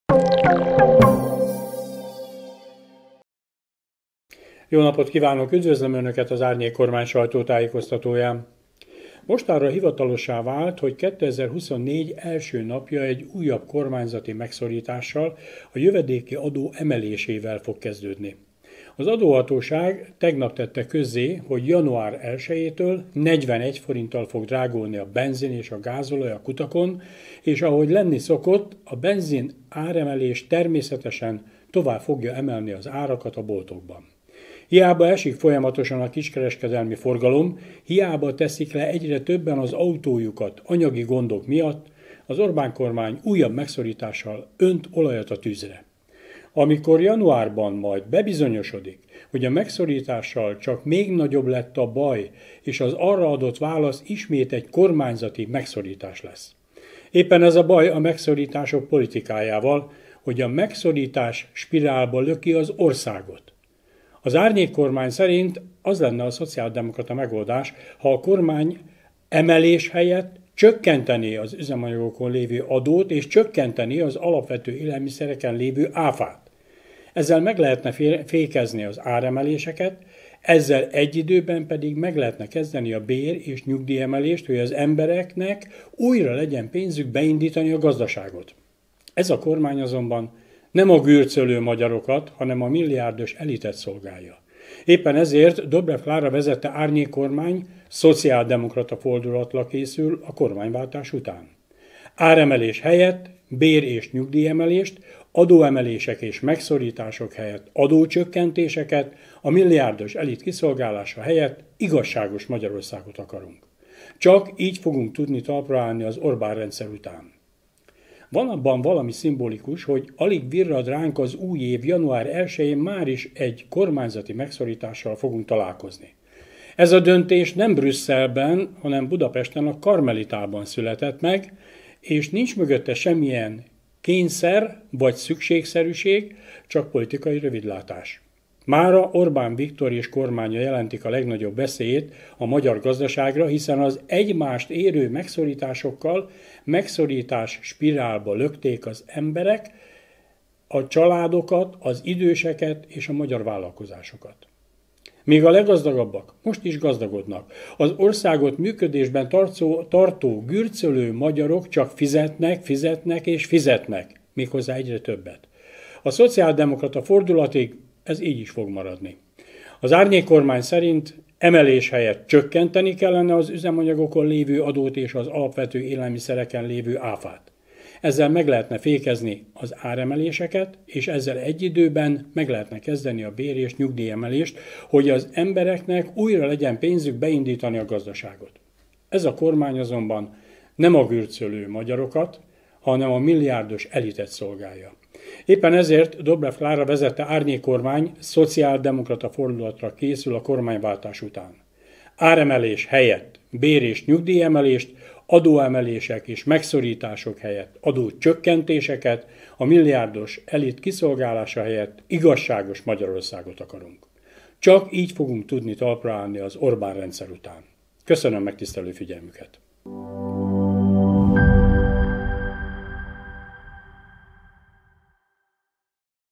41 forinttal drágul a benzin az Orbán-kormány jövedékiadó-emelése miatt!
Mostanra hivatalossá vált, hogy 2024. első napja egy újabb kormányzati megszorítással, a jövedéki adó emelésével fog kezdődni!

Az Árnyékkormány szerint az lenne a szociáldemokrata megoldás, ha a kormány emelés helyett csökkentené az üzemanyagokon lévő adót és csökkentené az alapvető élelmiszereken lévő ÁFÁ-t.

Kövesd élőben, és oszd meg Varju László energia- és rezsiügyi árnyékminiszter sajtótájékoztatóját!